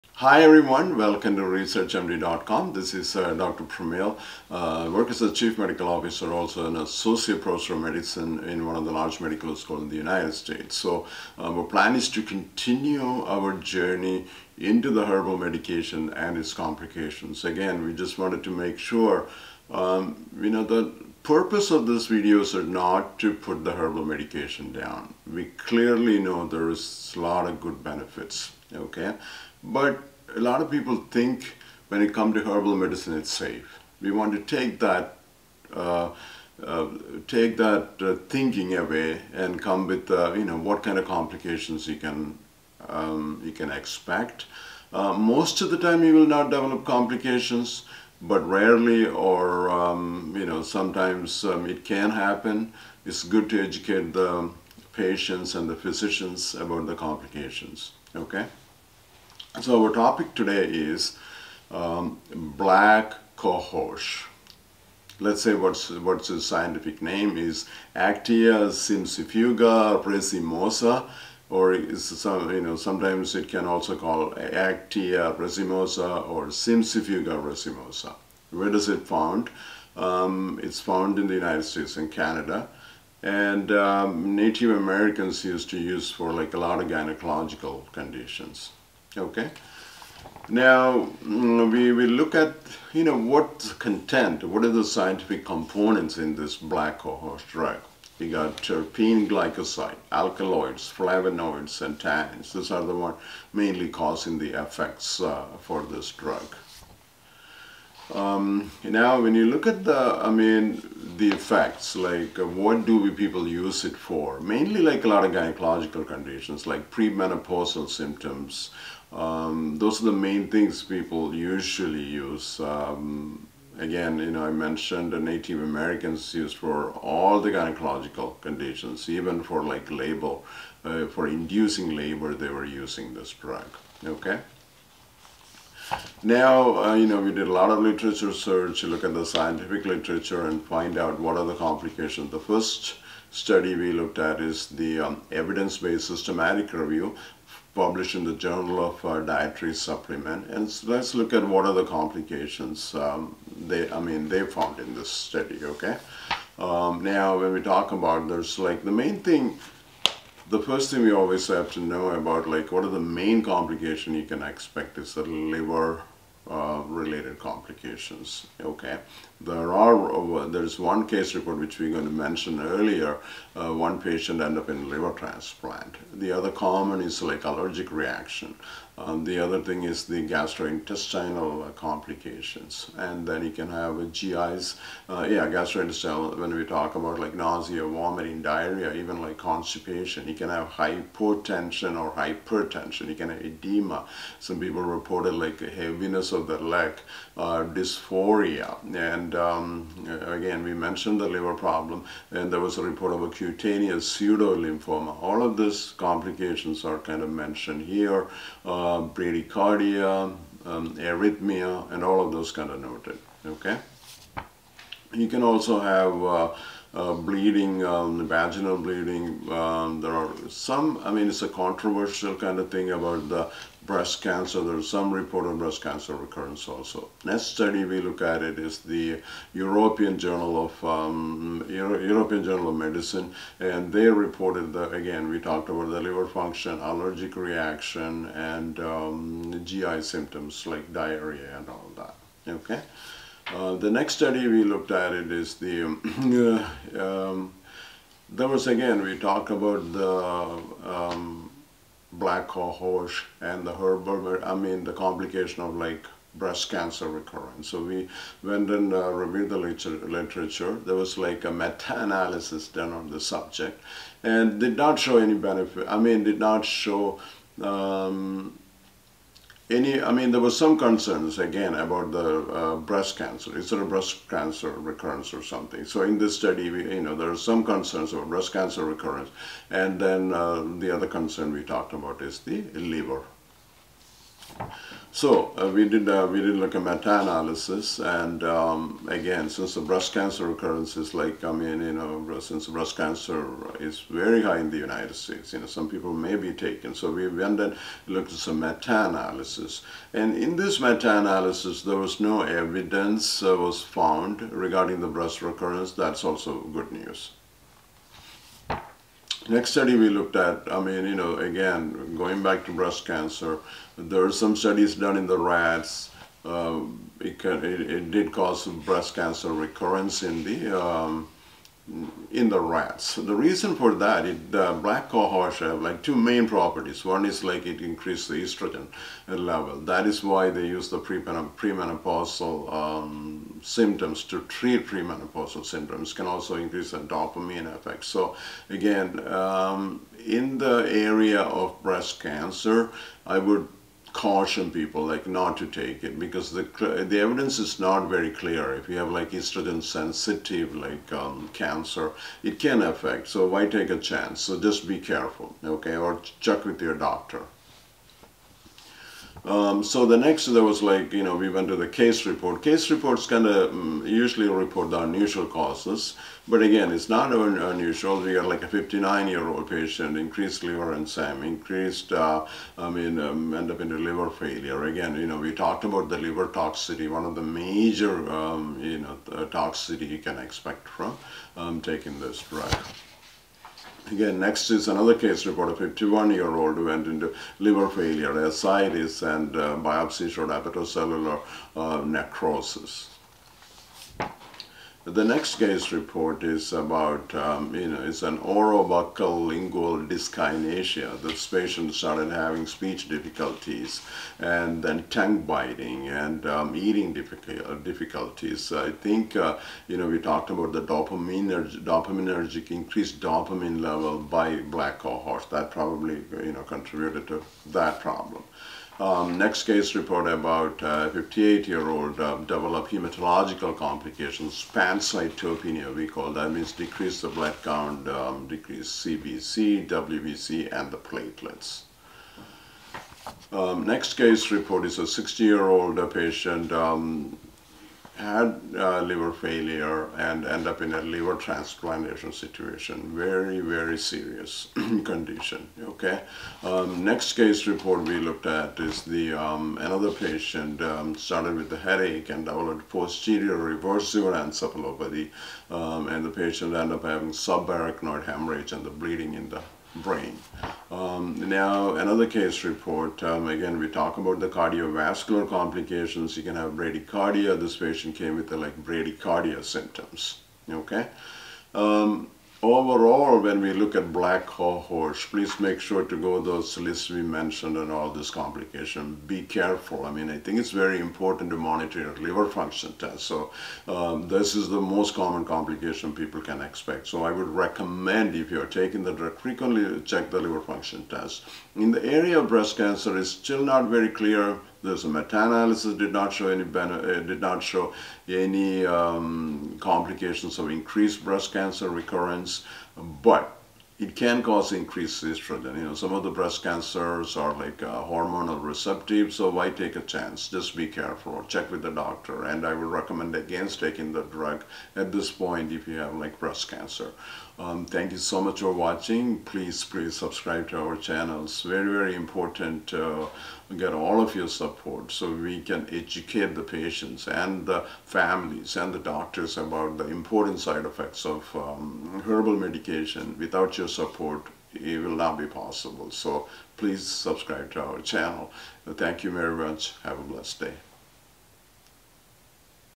Black Cohosh and its Toxic effects -scientific studies Dr Pramil, MD, MS, FACP
In this eye-opening video, we aim to shed light on the dark side of a popular medicinal herb known as Black Cohosh. Join us as we explore the toxic effects that lurk behind its seemingly beneficial properties.

Black Cohosh, scientifically known as Actaea racemosa, has been traditionally used to address various health concerns, particularly in women's health. It gained popularity for its potential to alleviate menopause symptoms, menstrual irregularities, and even support childbirth. However, it is crucial to understand that not all herbal remedies come without risks.

Through extensive research and expert insights, we will unravel the potential hazards associated with Black Cohosh consumption. We will explore its adverse effects, ranging from mild discomfort to severe complications, which have been documented in scientific studies and reported cases.

Discover the reported symptoms and side effects of Black Cohosh toxicity, including gastrointestinal distress, liver damage, allergic reactions, and potential drug interactions. Our video will provide a comprehensive overview of the scientific evidence available, ensuring you have accurate information at your fingertips.

To ensure balanced coverage, we will also discuss the limitations and controversies surrounding Black Cohosh research, highlighting the need for further studies to establish its safety profile definitively.

Whether you're a fan of herbal remedies or a curious skeptic, this video is essential viewing for anyone interested in understanding the potential risks associated with Black Cohosh. Empower yourself with knowledge and make informed decisions about your health.

Together, let's uncover the truth and navigate the delicate balance between natural remedies and potential toxicity.

This video is not for medical advice. It is very important to consult your physician for your medical problems.
Please consult with your physician if you have any medical problems.